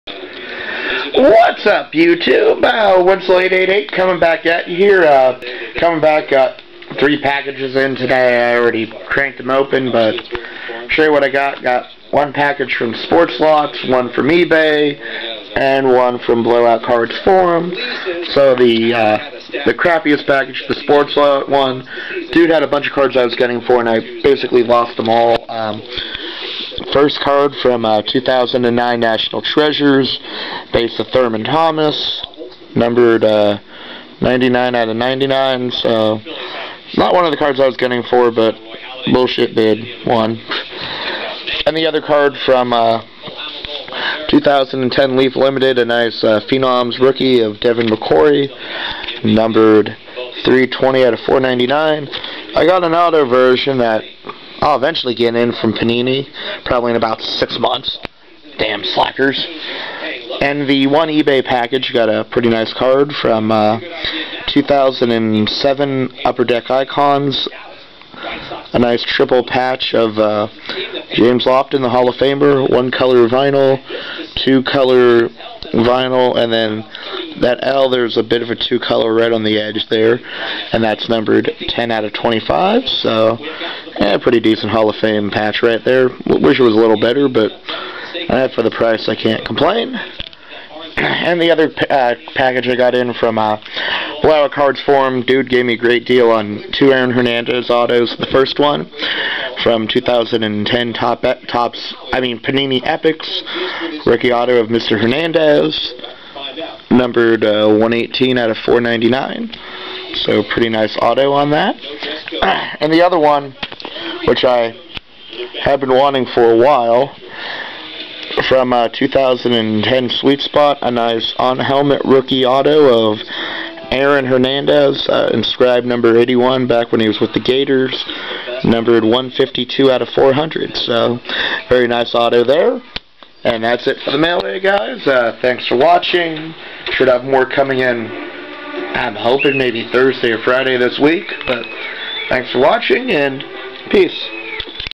Mailday from yesterday (Ebay dude, Blowoutcardsforum, Sportlots)
Hey guys, got a couple of packages in yesterday, nothing great but a couple of decent PC pickups, should have more hopefully come in before break starts on Friday.